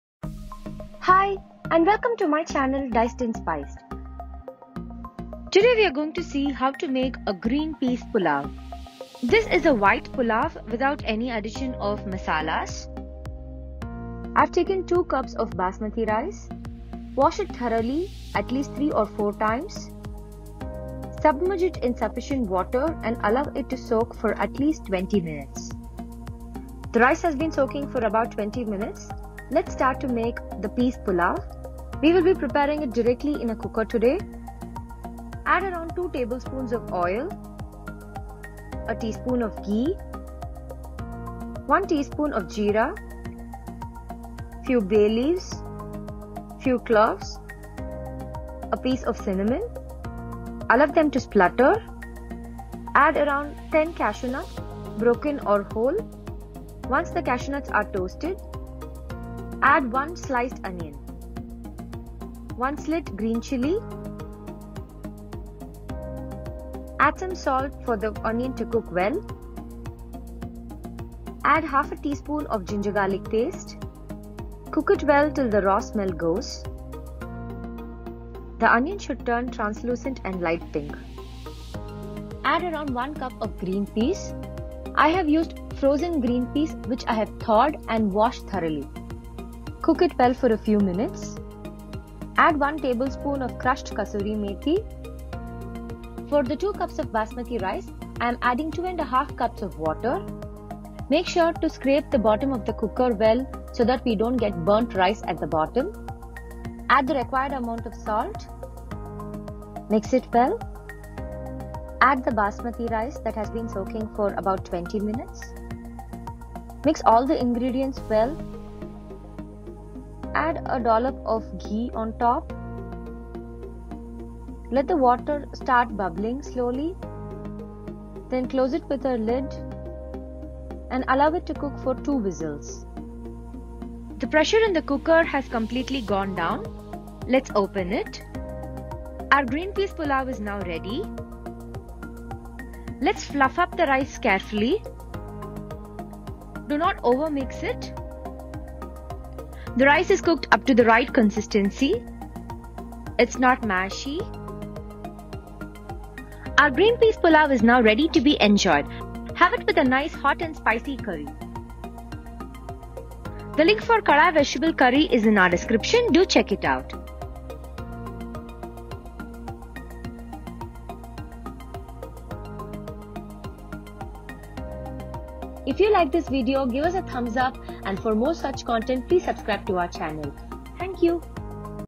Green Peas Pulao Recipe | One Pot One Shot Recipe | Quick & Simple Pulao Recipe
Green Peas Pulao

Ingredients:

Basmathi Rice - 2 Cups
Onion - 1 no
Green Chilies - 1 no
Green Peas(Frozen/Fresh) - 1 cup
Cloves - 3-4 nos
Bay Leaf - 1-2 nos
Cinnamon - a small piece
Cashews(Optional) - 8-10 nos

Serves : 3
Prep Time : 20 mins
Cook Time : 25 mins